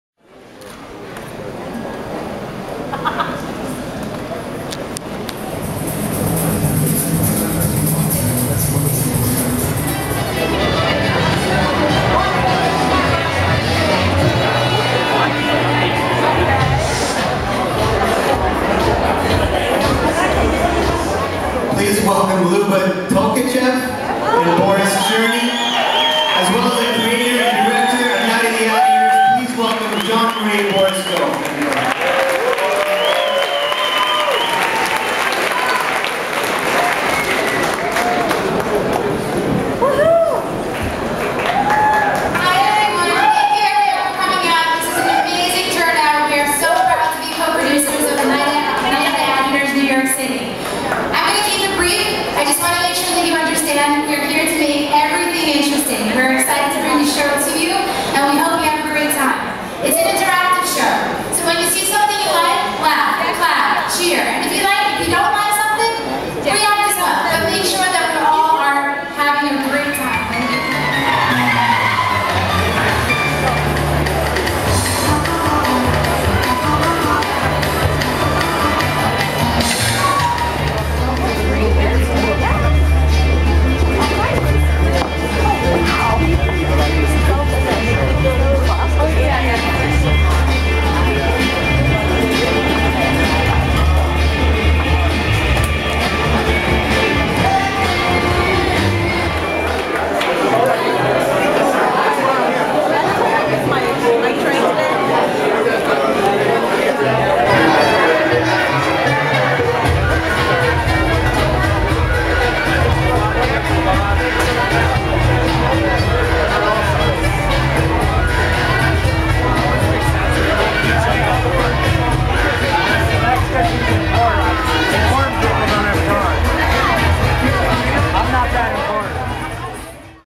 The Night of the Ad Eaters in New York City
The Night of the AdEaters® is an ALL NIGHT celebration of amazing, intriguing, shocking, and funny commercials, representing the vitality, creativity, passion and trends present in the world of advertising.